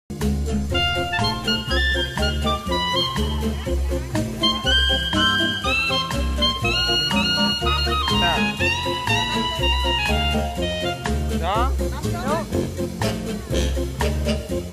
take a photo with elephant ...
take a photo with a big elephant. elephant so smart and lovely ..he is very strong...